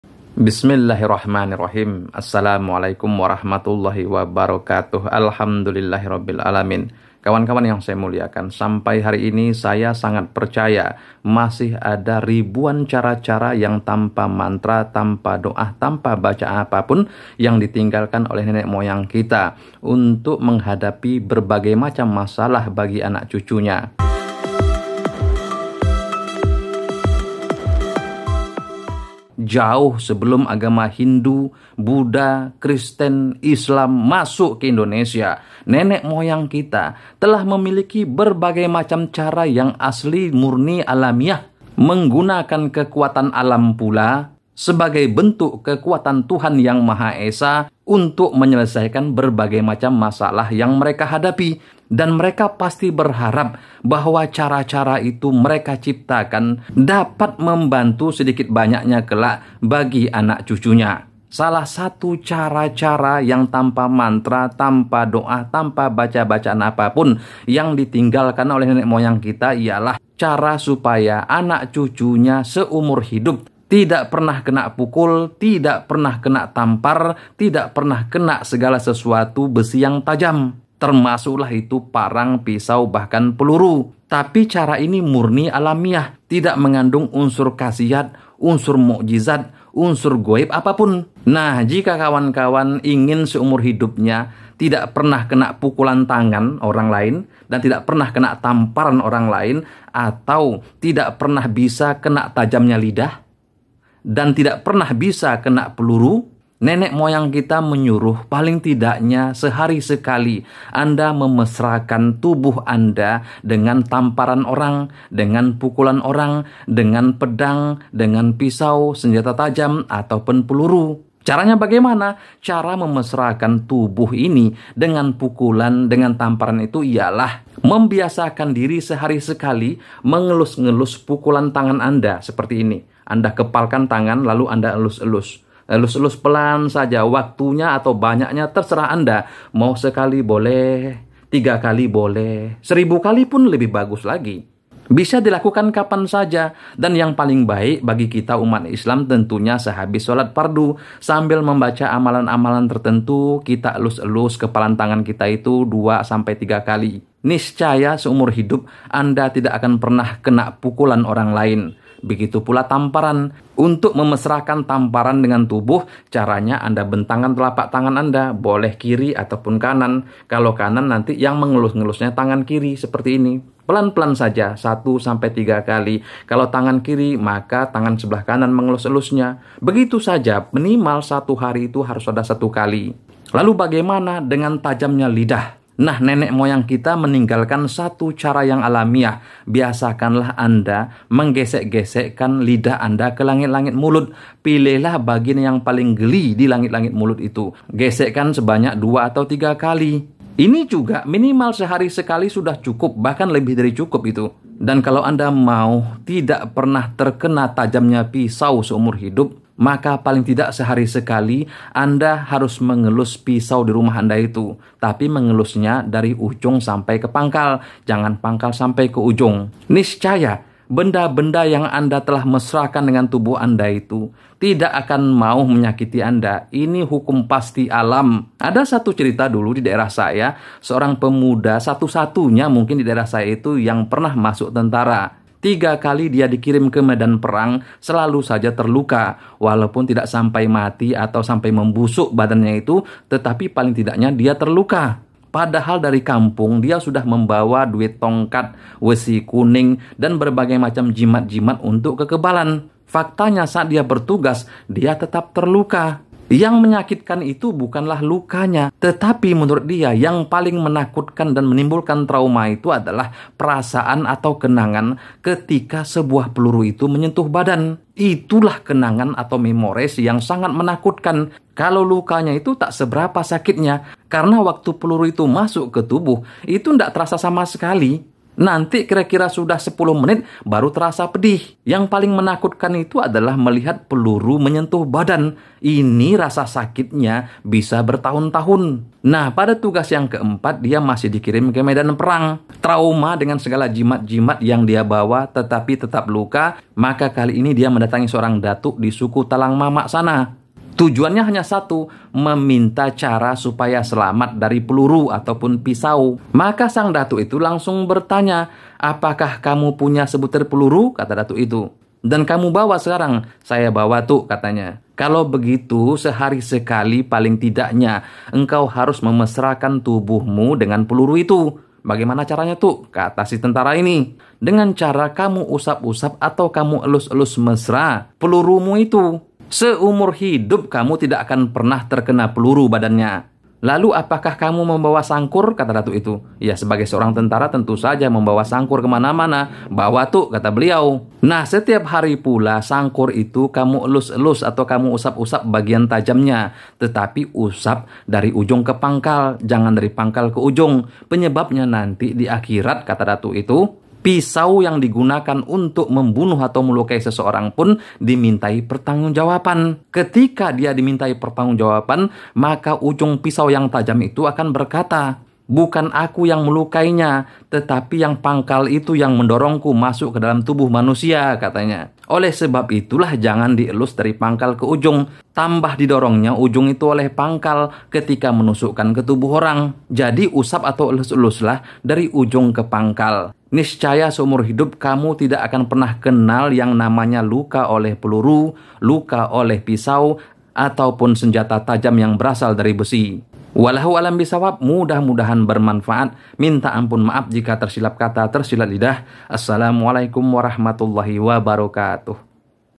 Keajaiban Mengusap Peluru
tampa mantra tampa doa anda akan kagum dengan warisan nenek moyang kita ini